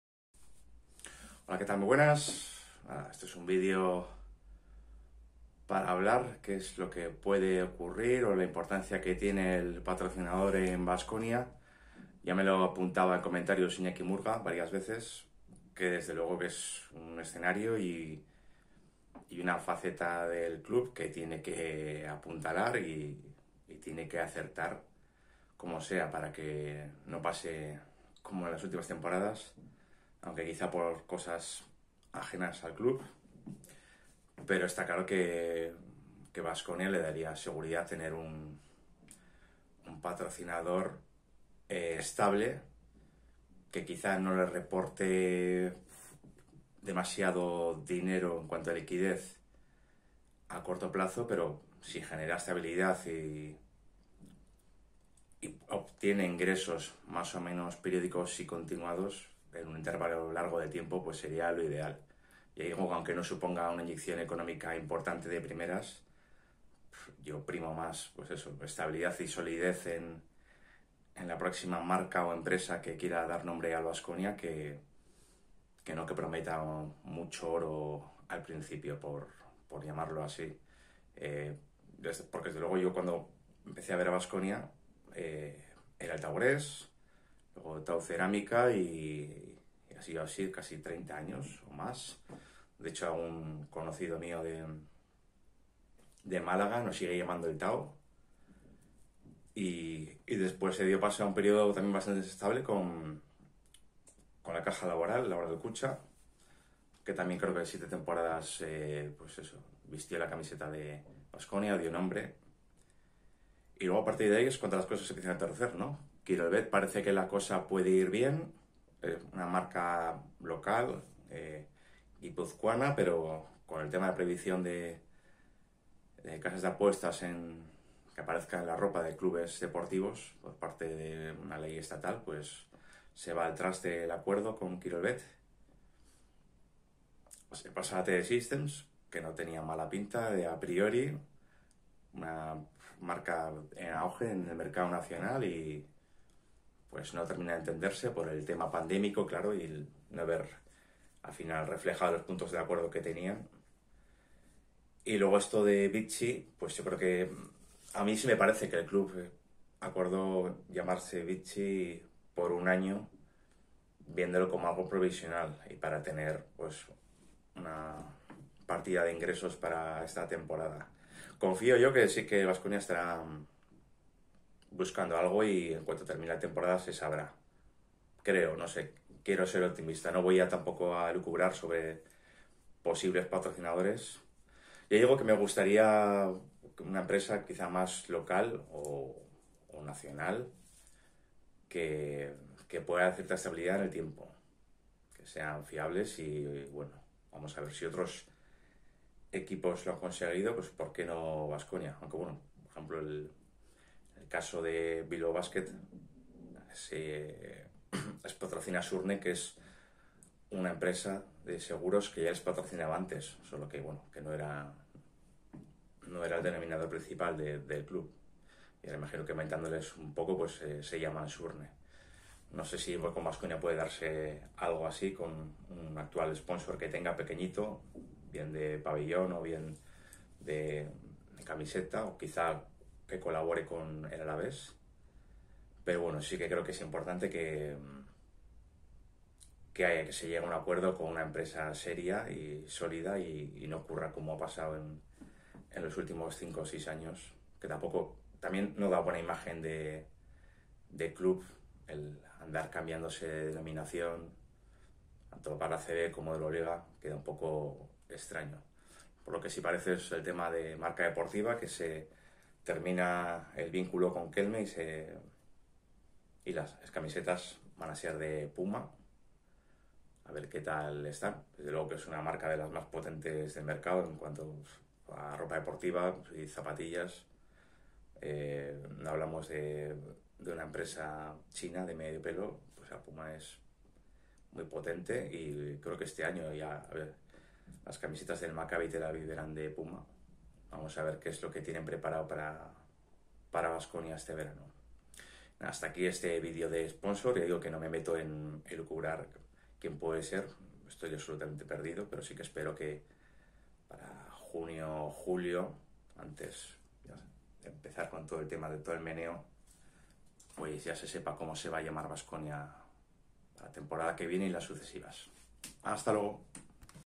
ACERTAR CON EL SPONSOR DEBE SER CAPITAL PARA BASKONIA
Baskonia debe acometer este verano un cambio de sponsor que garantice cierta estabilidad para sus arcas